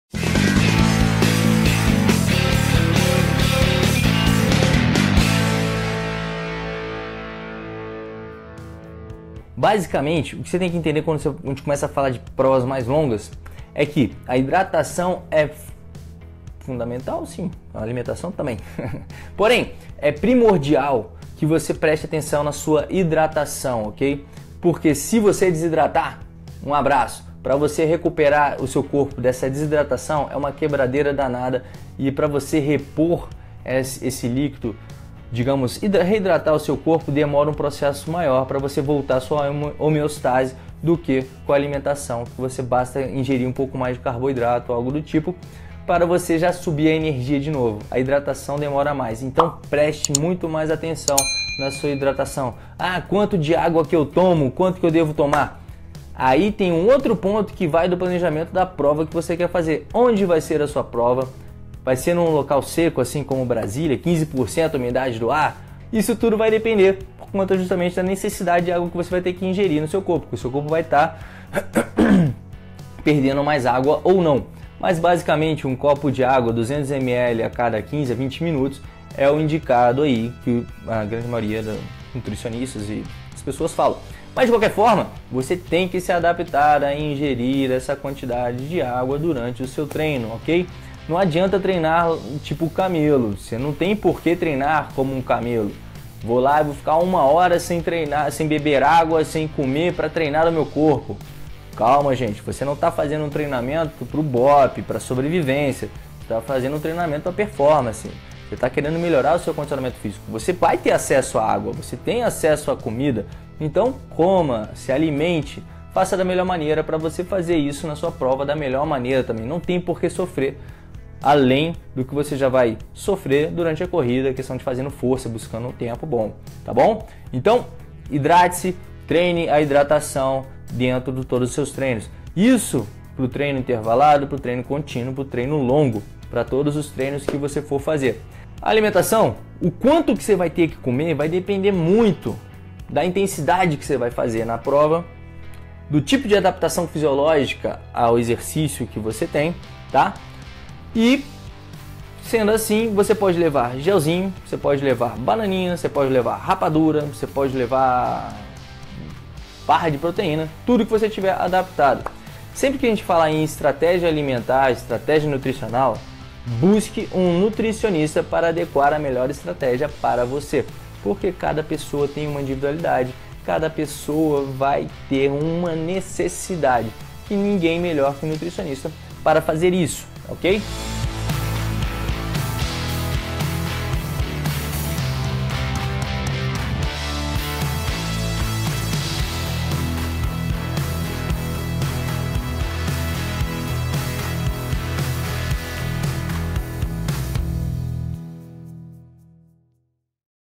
Cuidado com a desidratação na corrida! 🥵
Como evitar a desidratação na corrida? Qual quantidade de água ingerir? Devo me preocupar mais com a hidratação ou com a alimentação na corrida? Entenda mais sobre o assunto neste vídeo rápido.

Prepare o seu corpo para a corrida. Experimente os exercícios Grátis do nosso método. Clique aqui »